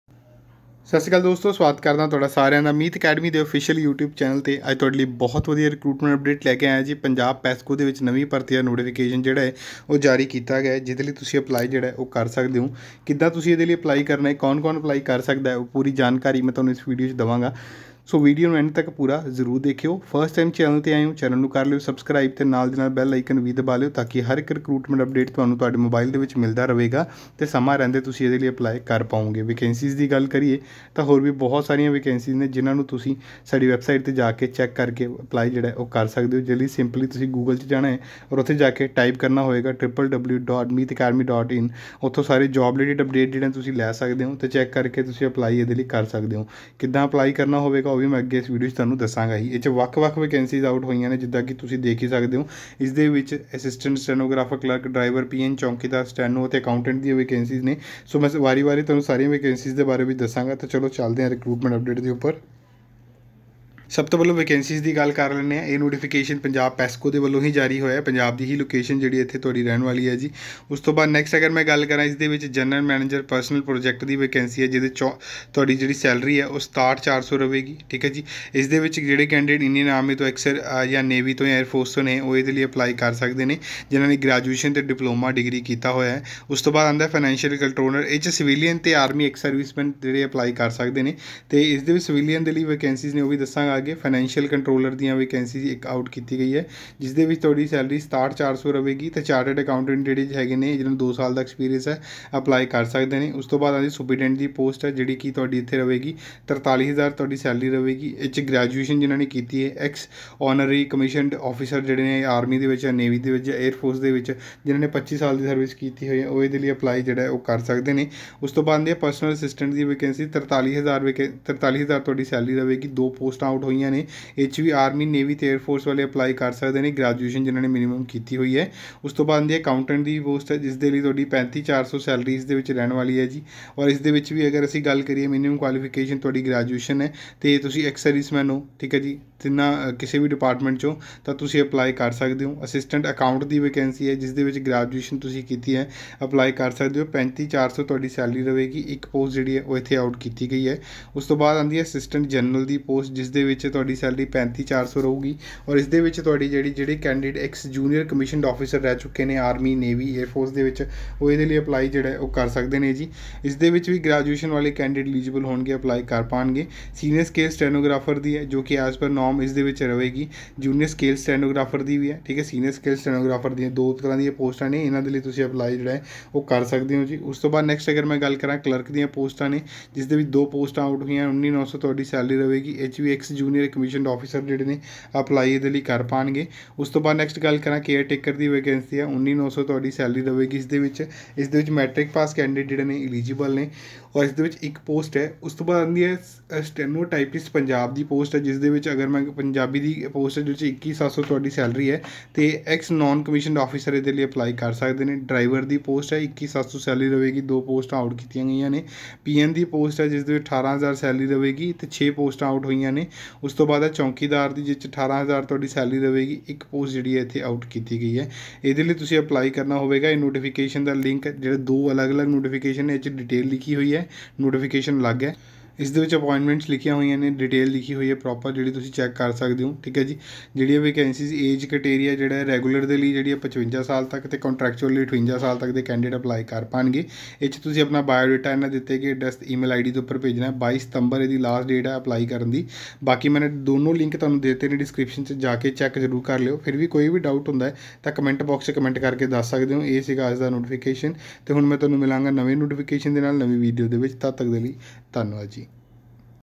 ਪੰਜਾਬ ਵਿੱਚ 3520 ਪੋਸਟਾਂ ਆਈਆਂ| Punjab PESCO Recruitment 2024 | Punjab Govt Jobs Sep 2024| Meet Academy
Govt Jobs Best Study Material 👇

1.

2.

3. Punjab Patwari Agriculture(Top 500 MCQ)

4. Best Punjab GK Book for all Punjab Govt Exams-Pdf